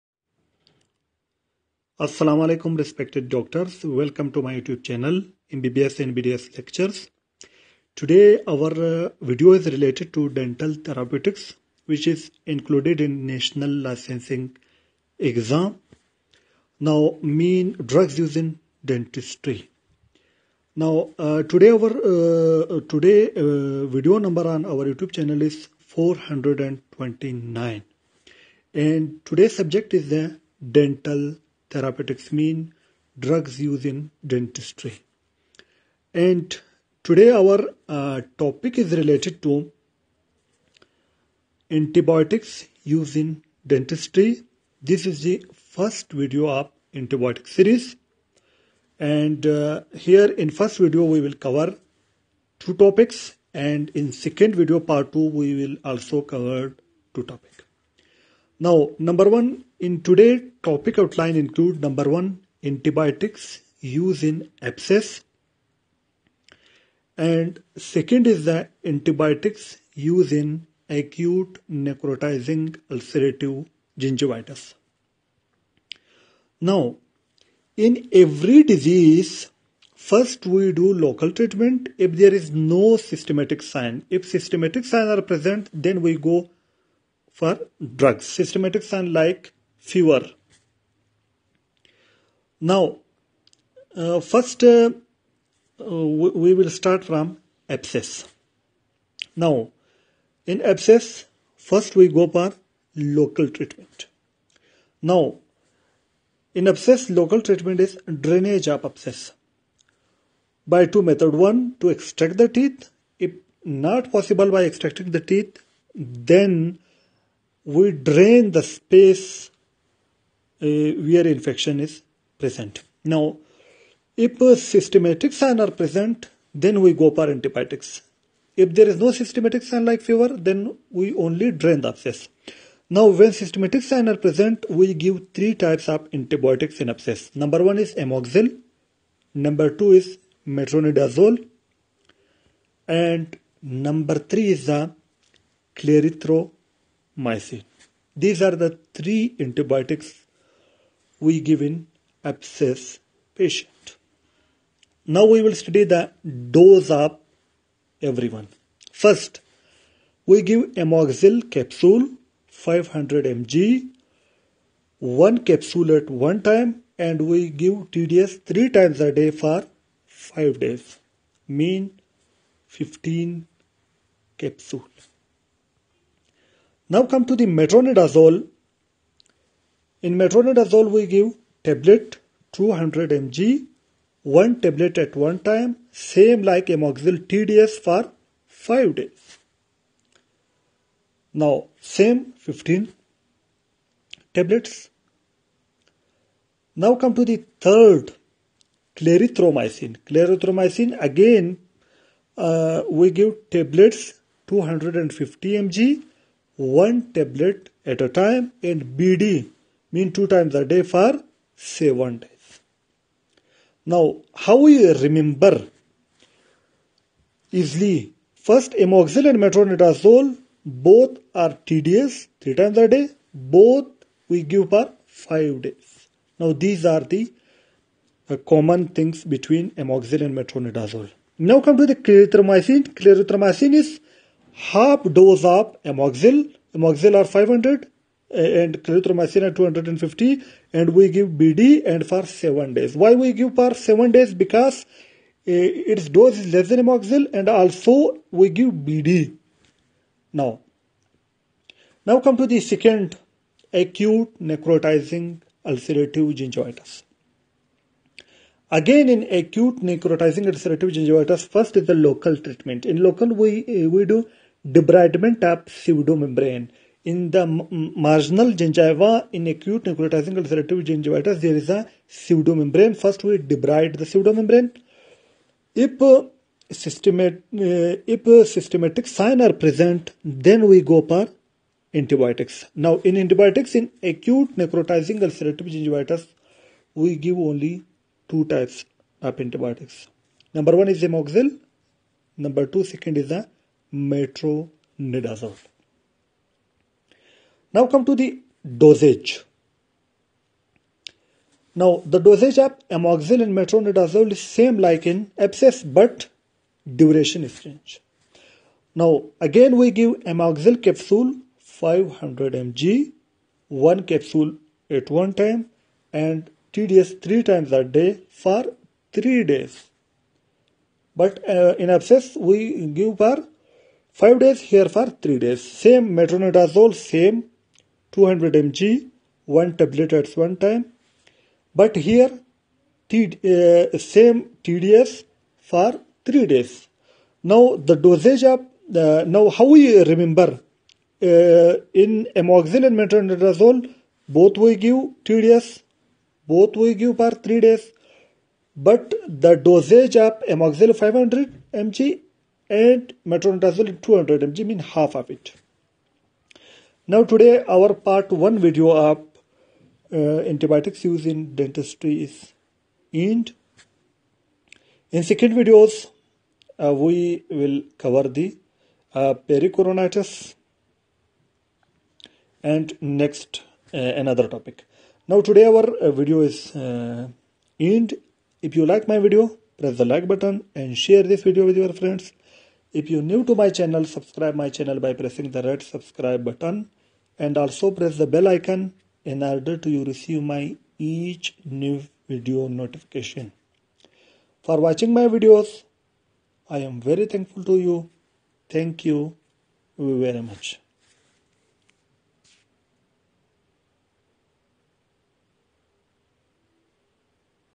Antibiotics used in Dentistry|Drugs use inDentistry|Antibiotic use In ABCESS/ANUG|Dental Therapeutic
This is the first video of Dental Therapeutics in which I explain the antibiotics use in Dentistry part 1